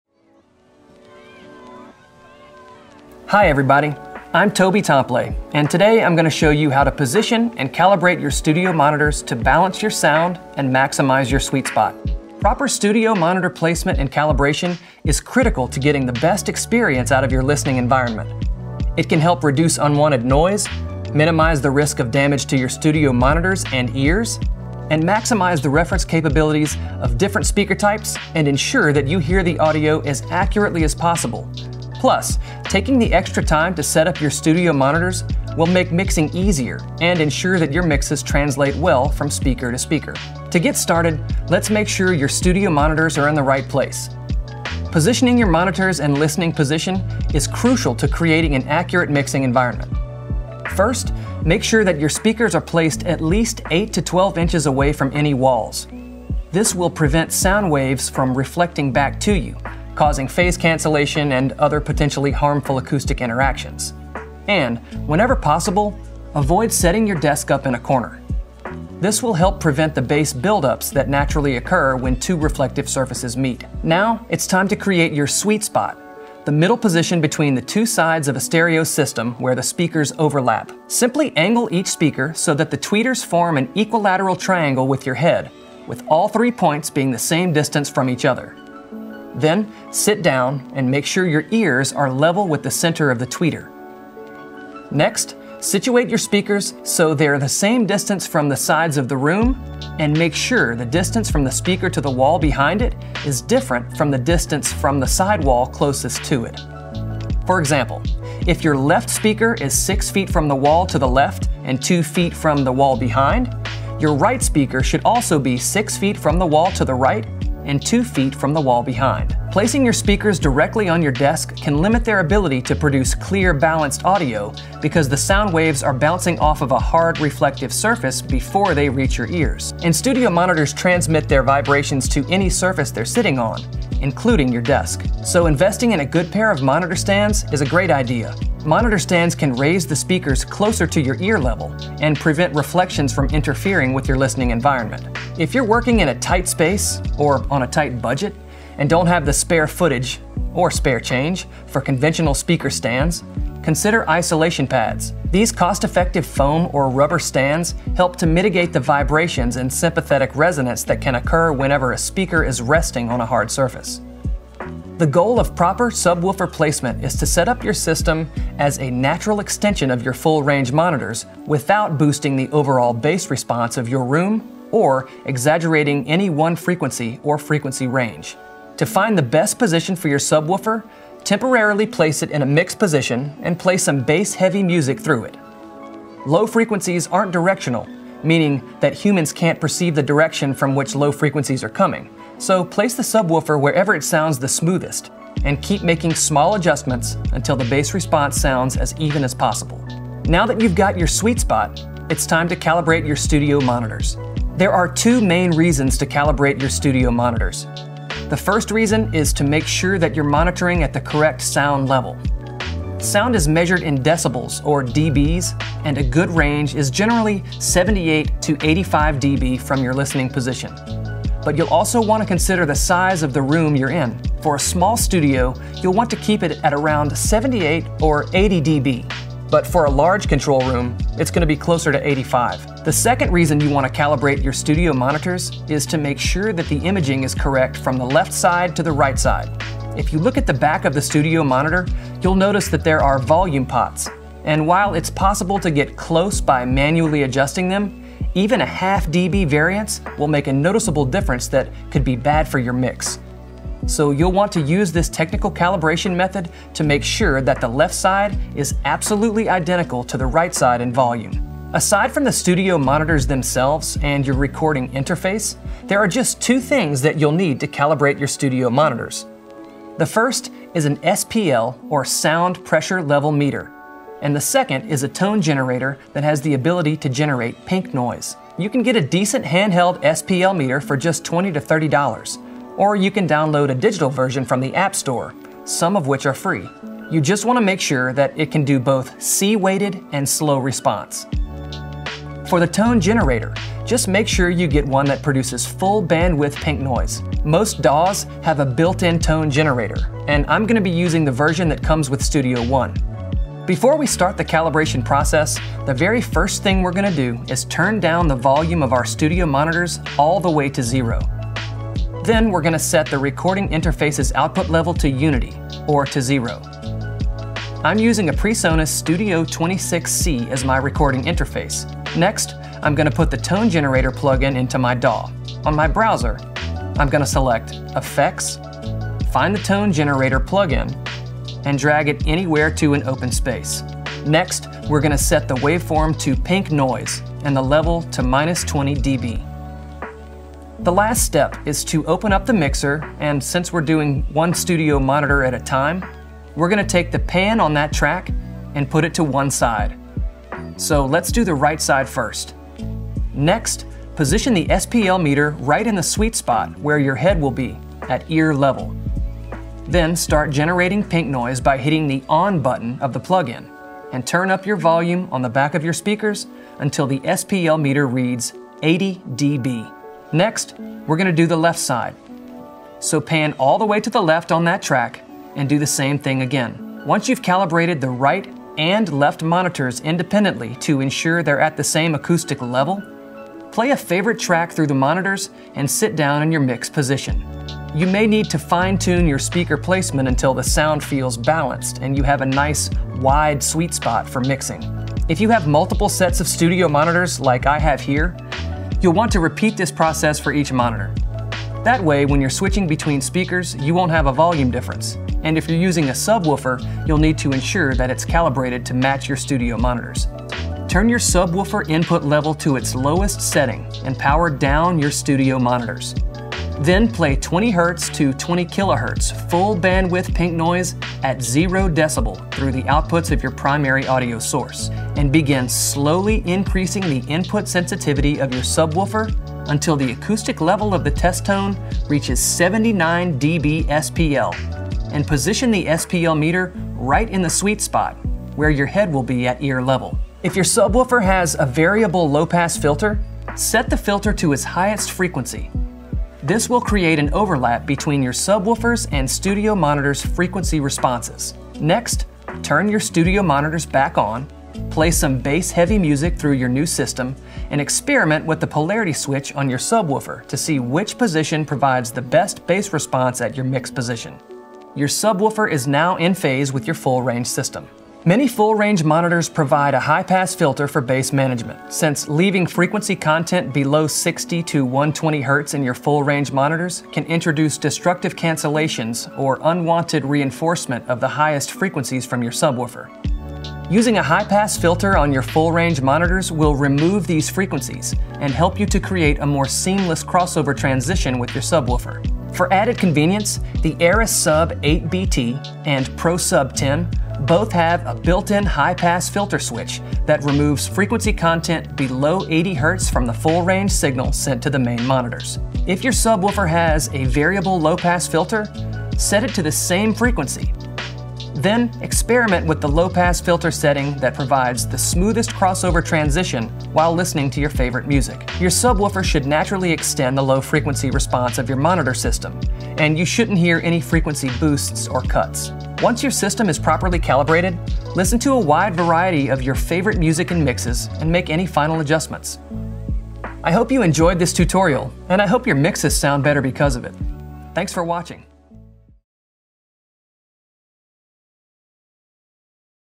How To Calibrate Your Studio Monitors | PreSonus
Proper studio monitor placement and calibration is crucial to getting the best listening experience from your environment. Taking the time to do this correctly will make mixing easier and can help ensure that your audio mixes translate better from speaker to speaker.

In this video, we'll help you achieve the best results by guiding you through the following:

Step 1: Positioning your studio monitors
Step 2: Positioning your subwoofer
Step 3: Calibrating your studio monitors
Step 4: Calibrating your subwoofer